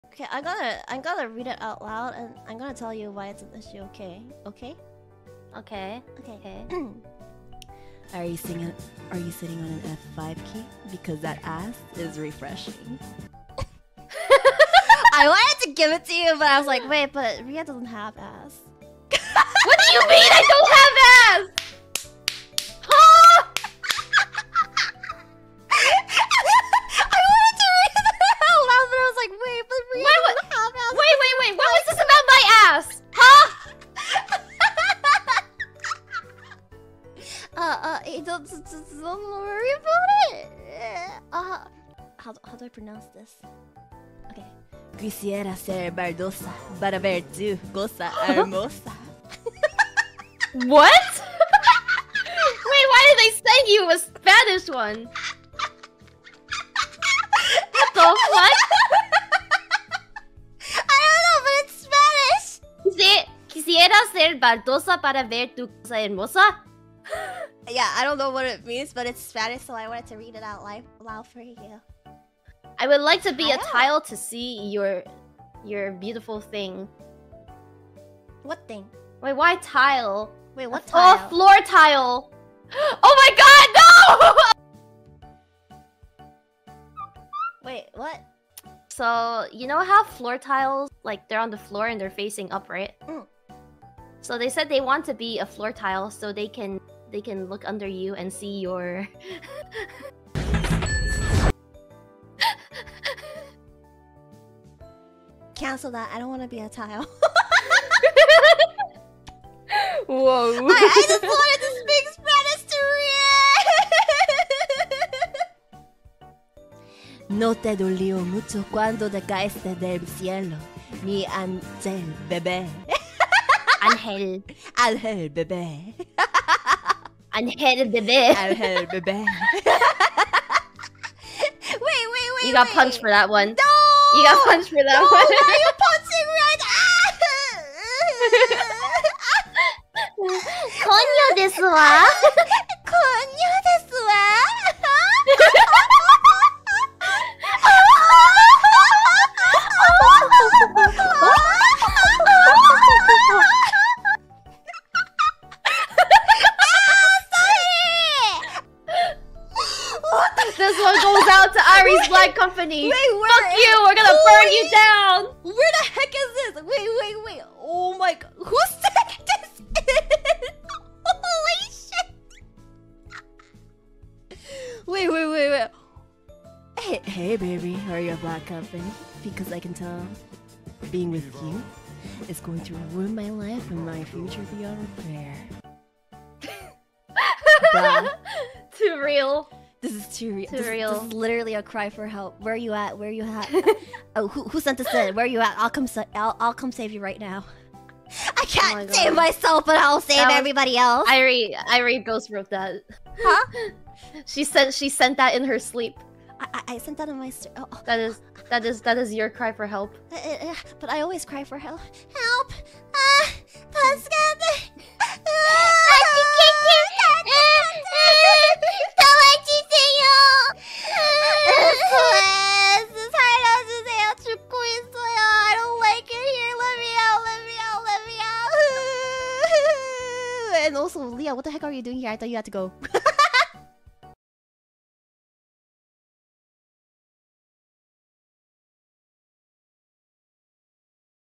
Airi reads cringe pickup lines
The whole stream was fun, make sure to watch it.